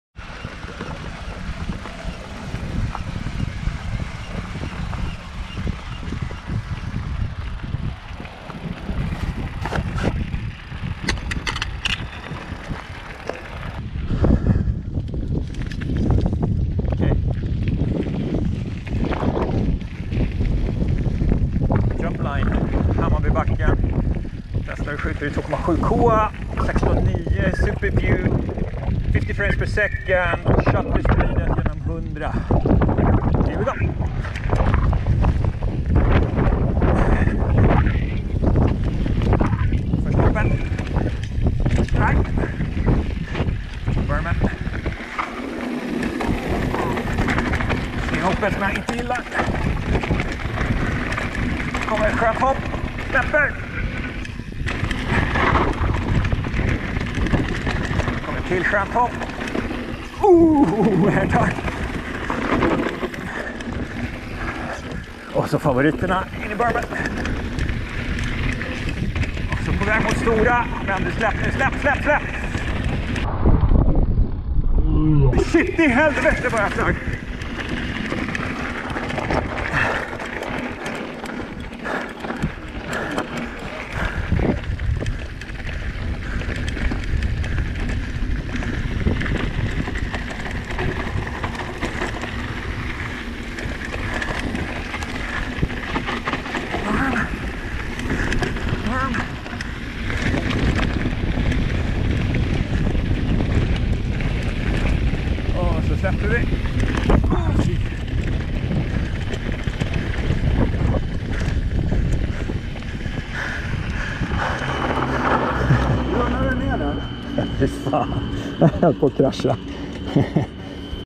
Hammarby Bike Park
Jump-line en magisk sensommarkväll. Golden Hour med både super smooth på GoPro Hero 7 och FeiyuTech WG2 gimbal.